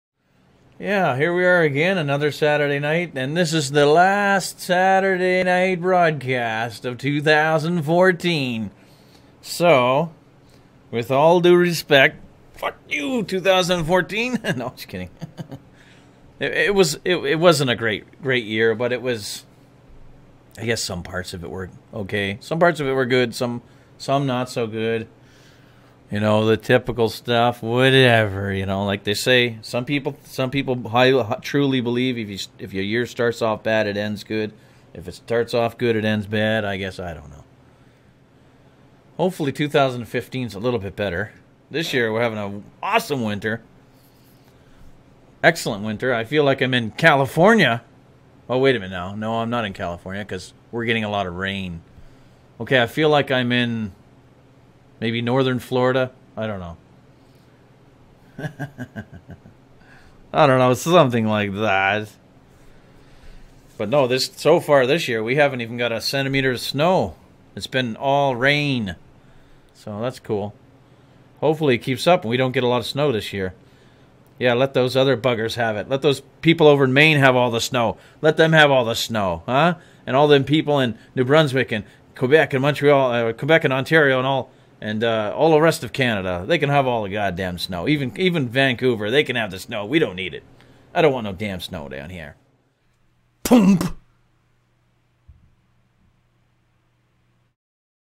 Jake got Punked!!!
From very early Jan. to early April 2015 we recieved nearly 2 meters or about 6.5 feet of snow total! Guess I shoulda kept my mought shut! LOL